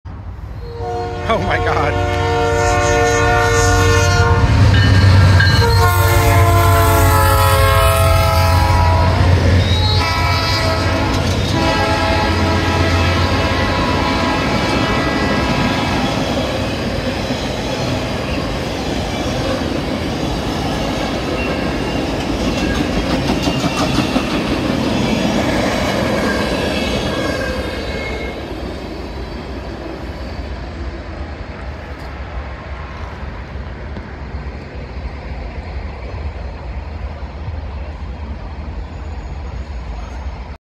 Catching Westbound NS 955 Office Car Special a 2nd time, crossing E. Wyoming Ave, bound for Gest St
Lockland, Ohio MM250, Tuesday, August 13th, 2024 about 10:08 AM.